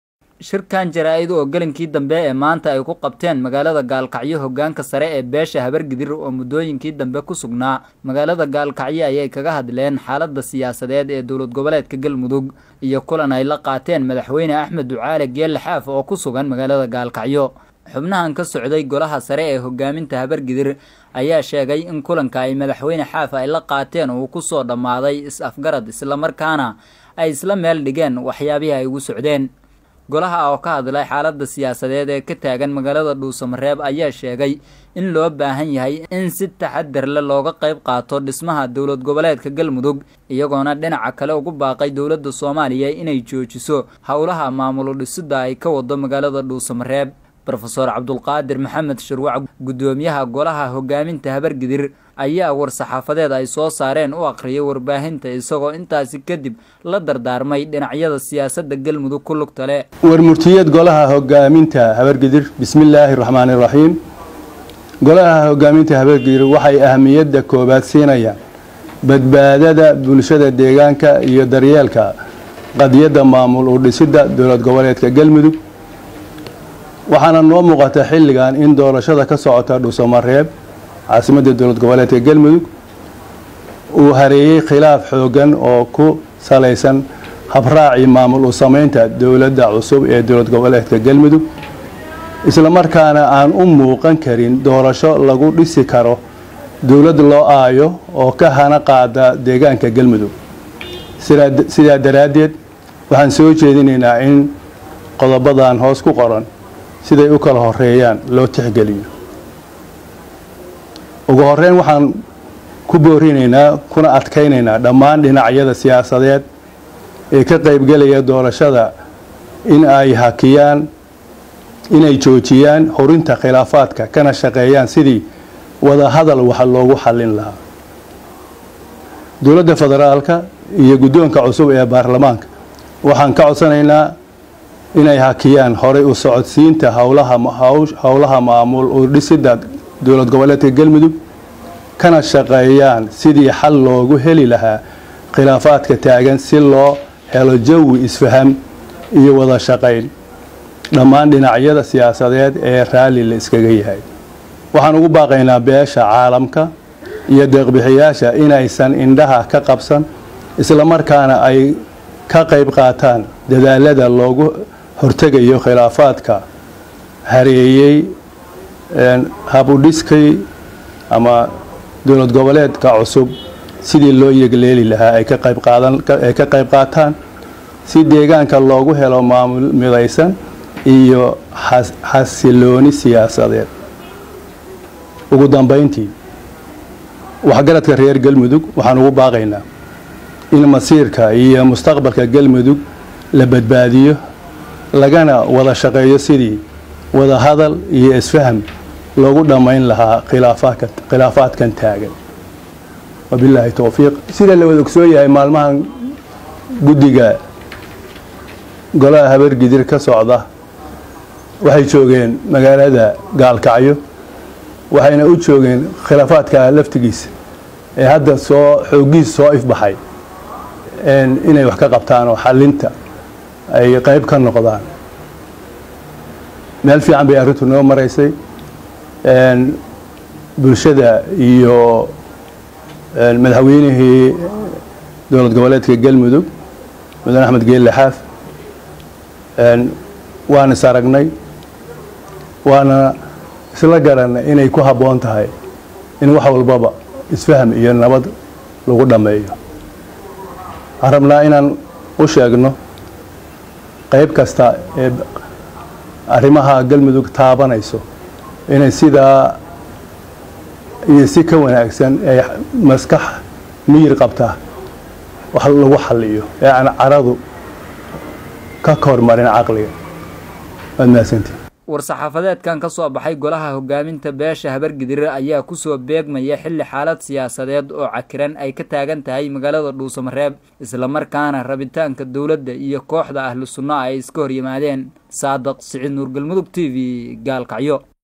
Kulan xal raadin ah oo ka dhacay Magaalada Gaalkacyo
Golaha Hogaaminta Habargidir oo sheegay inuu is afgarad ku soo dhamaaday kulan ay la qaateen Madaxweyne Xaaf